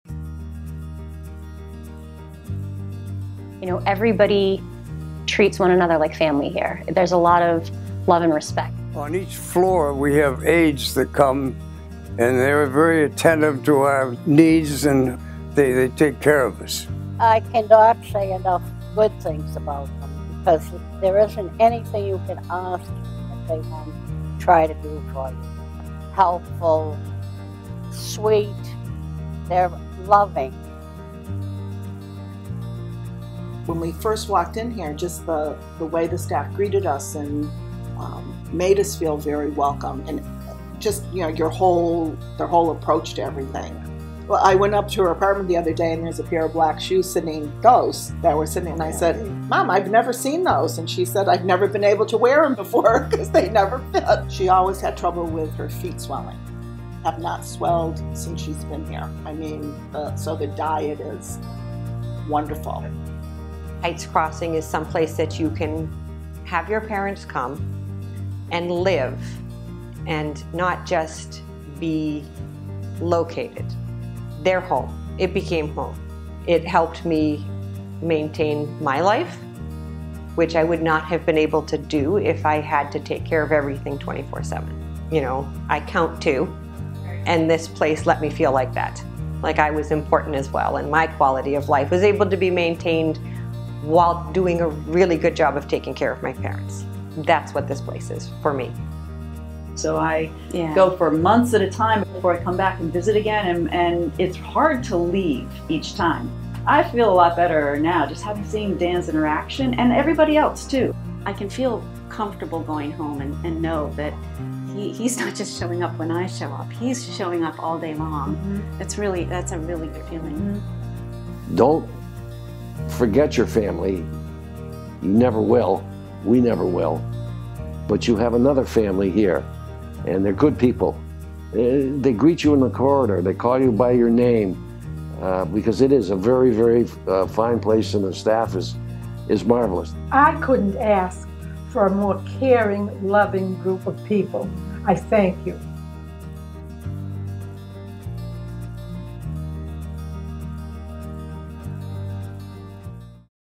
Senior Living Residences Family Testimonials
The best way to learn about Senior Living Residences (SLR) is to hear how real residents, their families and associates talk about us. We are fortunate to have so many willing to share the positive experiences they've had at SLR communities, and we sincerely thank them.